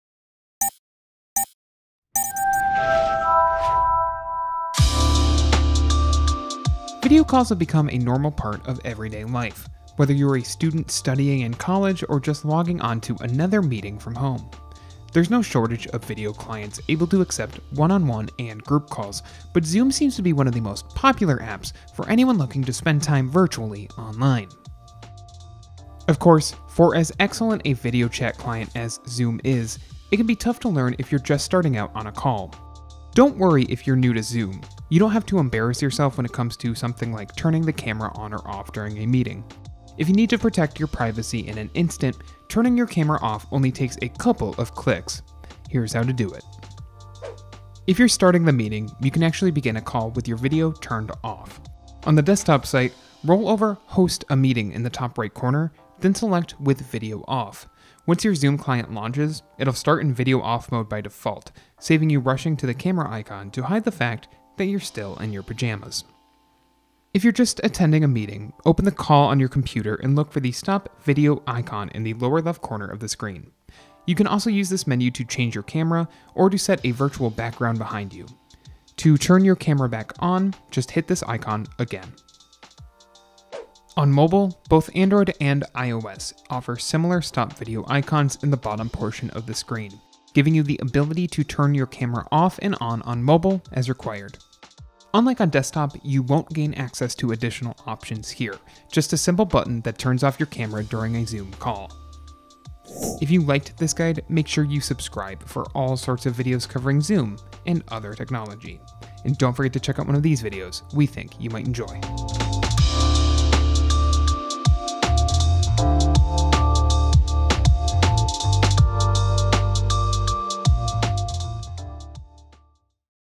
How to Turn Off Your Video Camera During a Zoom Call
Video calls have become a normal part of everyday life, whether you’re a student studying in college or just logging onto another meeting from home. There’s no shortage of video clients able to accept one-on-one and group calls, but Zoom seems to be one of the most popular apps for anyone looking to spend time virtually online.